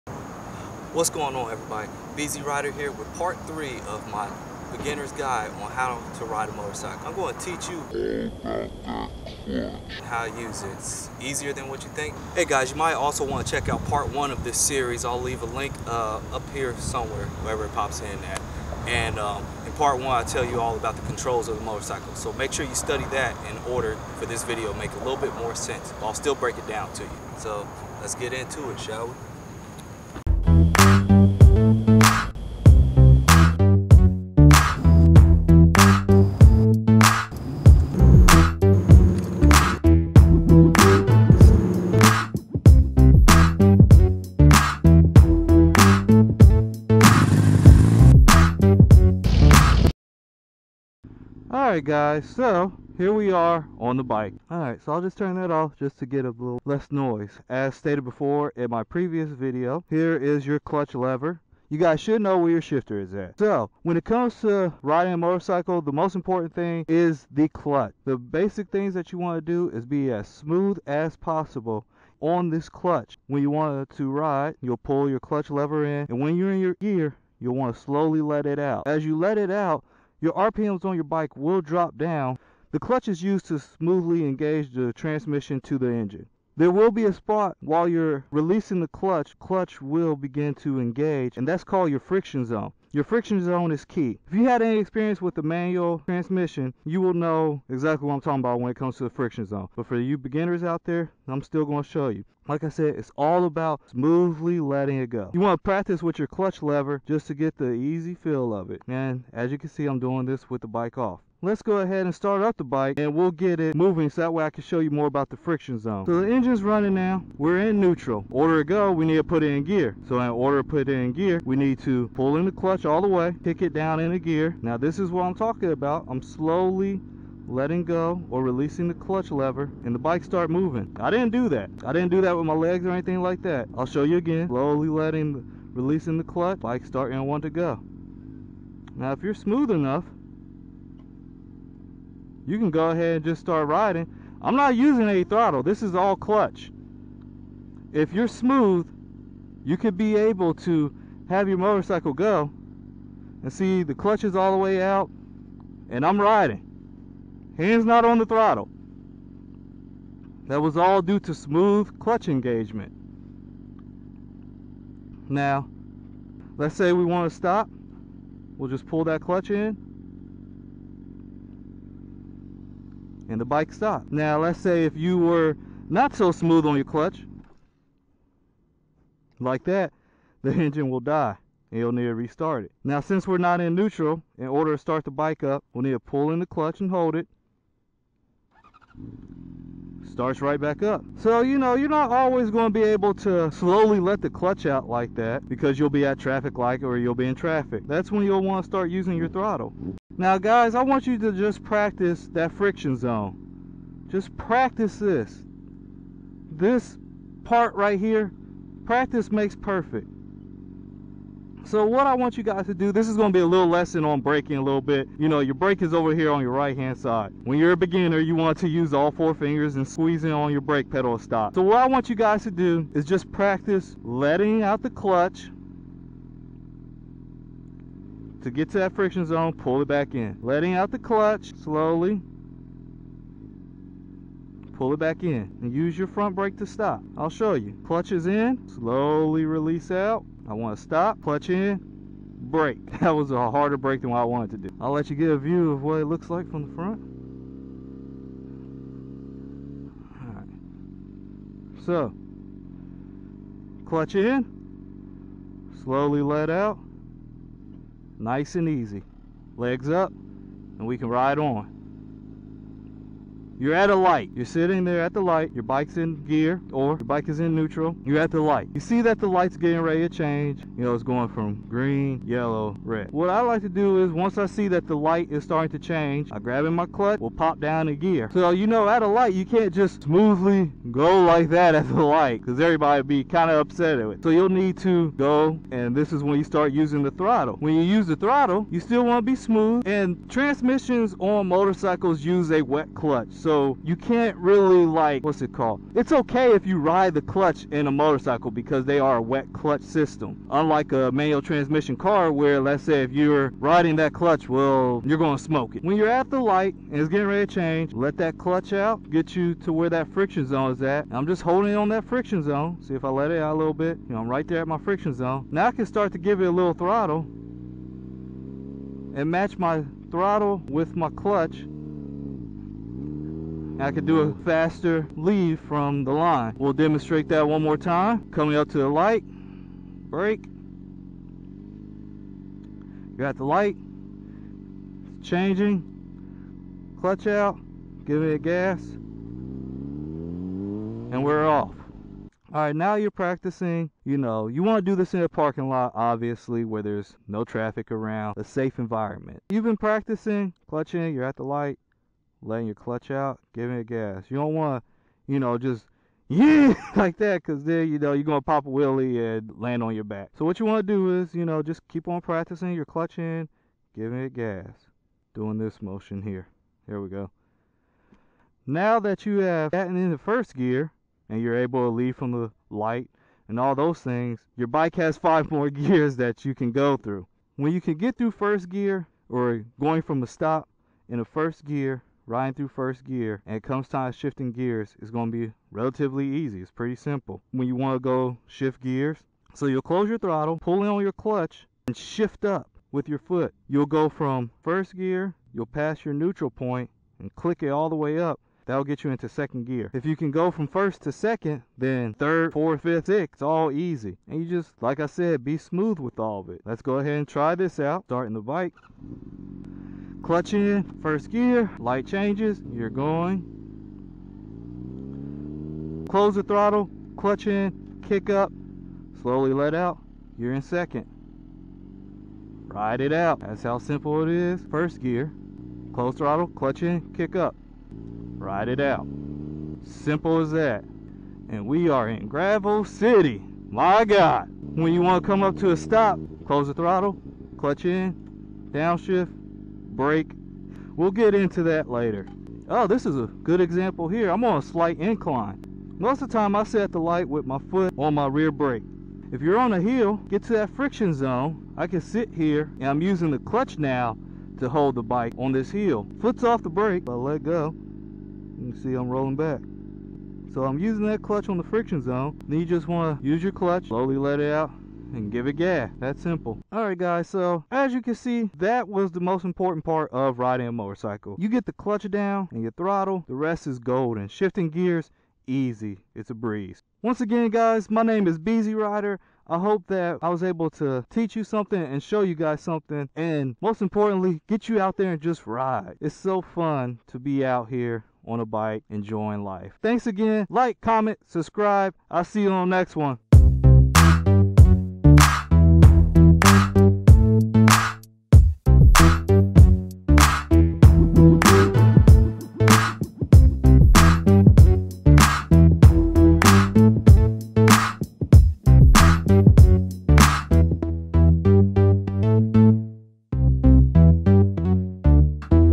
How to Ride a Motorcycle for Beginners Part 3 Clutch Control and Shifting
Alright now its time to learn how to use the clutch and start going on your bike!

In this video, I break down how to use smoothly engage your clutch that way you can get your bike moving.

Learning how to ride a motorcycle can seem complicated but its easier than you think.

Practice, practice, practice and you'll go from a beginner, to a rider in no time!
It's easier than you think so sit down, take notes, and get out there and RIDE!

Bike: 2007 Honda CBR600rr

Camera: Nikon D3200, ActivEon Action Cam